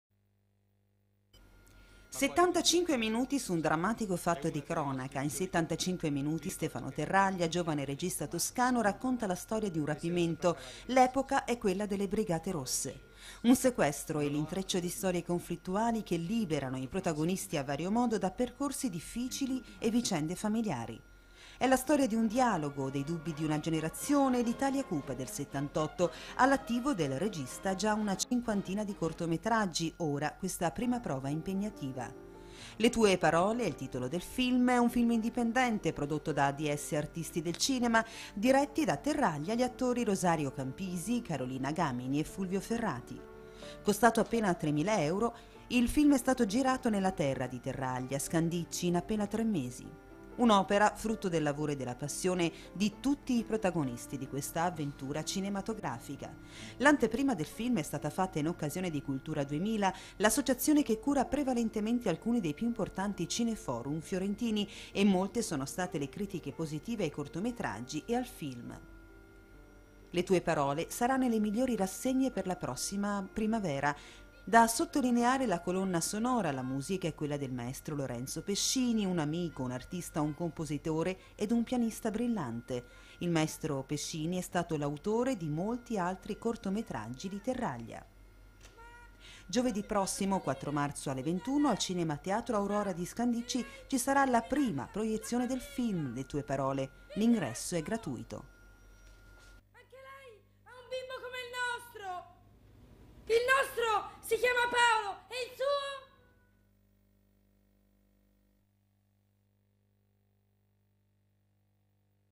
Le tue parole - Granducato TV.mp4
Ecco l'interessante servizio sul film di Stefano Terraglia, "Le tue parole", realizzato dalla bella e brava Anna Romano e trasmesso sui TG della emittente televisiva Rete Granducato TV, nel Marzo 2010. La colonna sonora del film è del maestro Lorenzo Pescini, gli interpreti sono Rosario Campisi, Carolina Gamini, Fulvio Ferrati. Il direttore della fotografia è Nicola Serena, l'assistente alla regia è Giulia Spagnesi. Dopo la prima visione al Cinema Aurora di Scandicci (4 marzo 2010) le repliche al Cinema Castello di Firenze (22 e 23 aprile 2010)